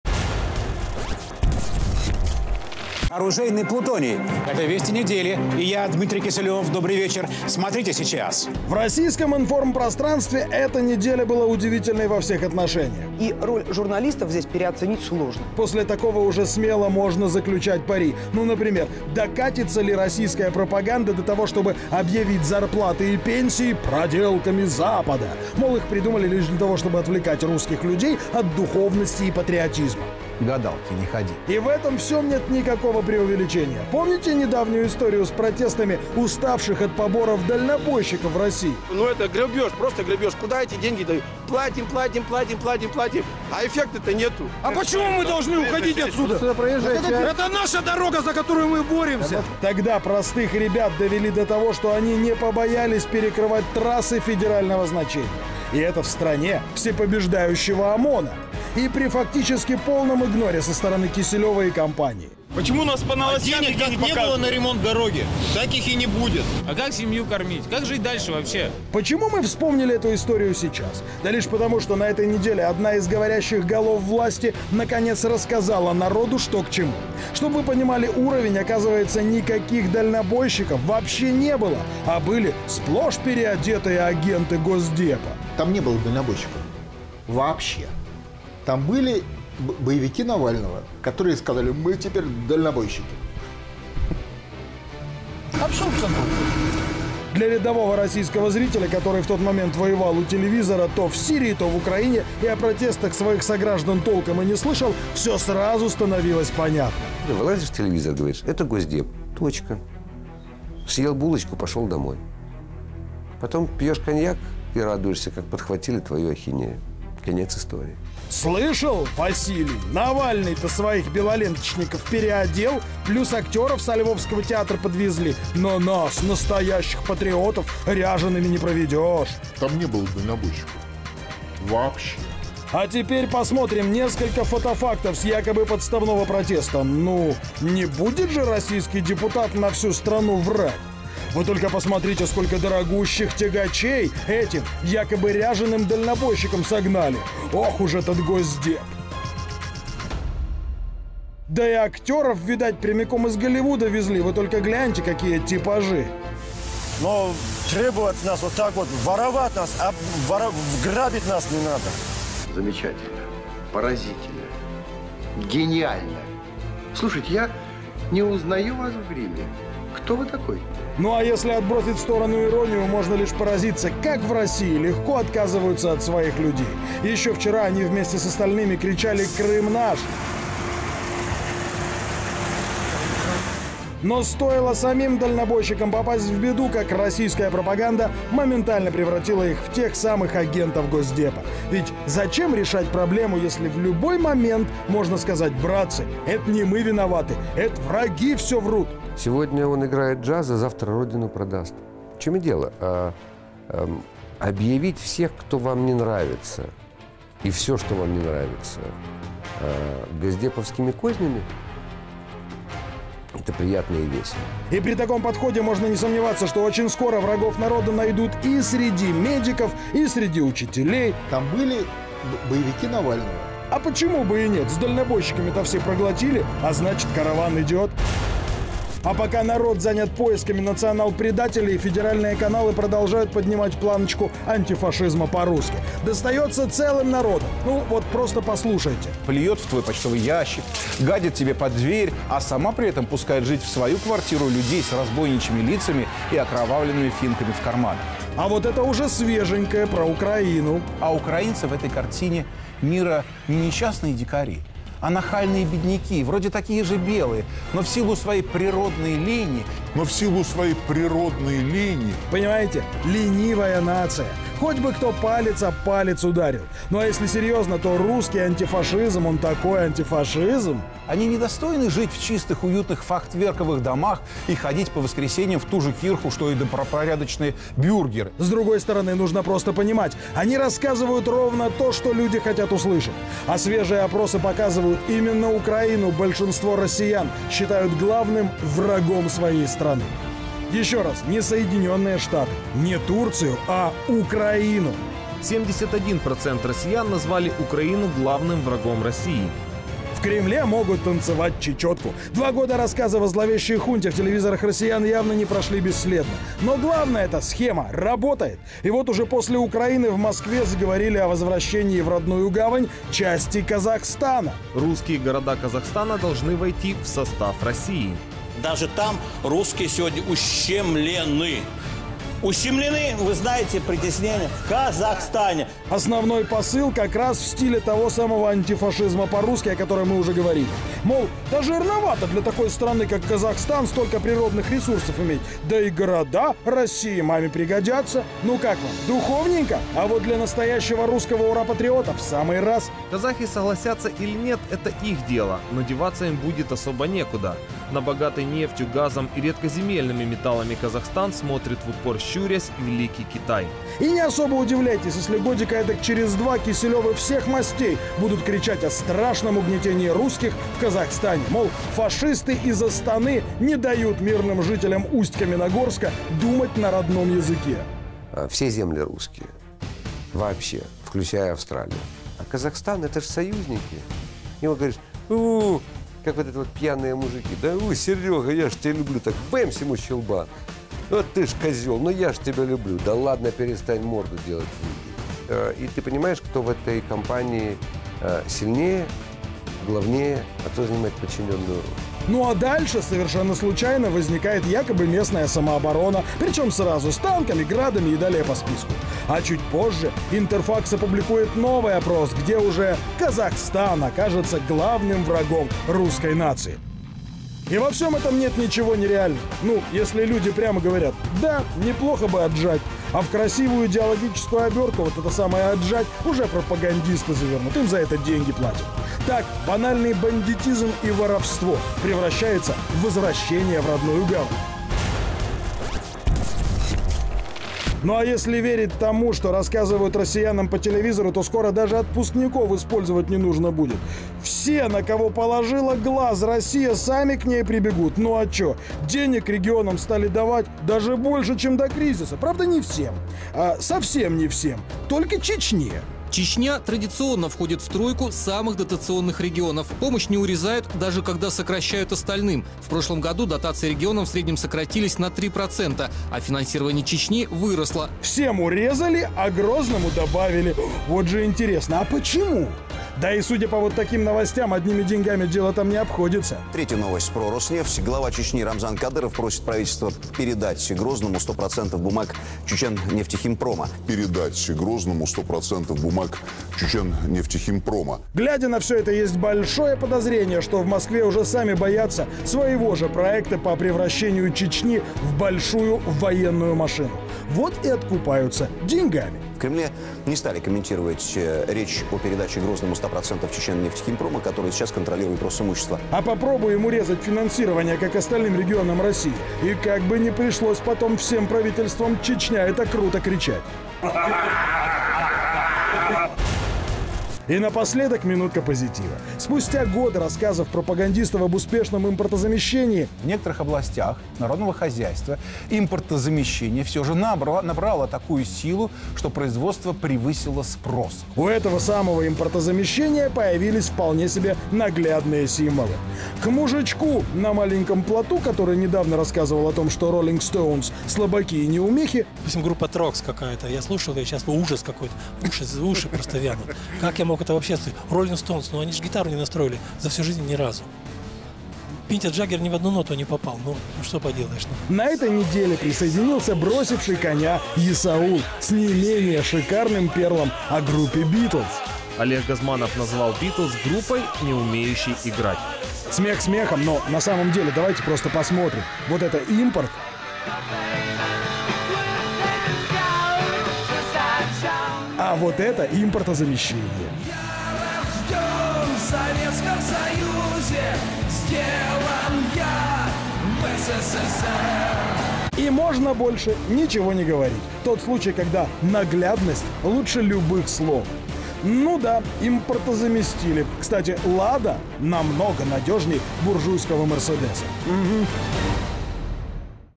Как российские СМИ скрывают не выгодную Путину правду — Антизомби, 15.04
В Российском информпространсве эта неделя была удивительной во всех отношениях.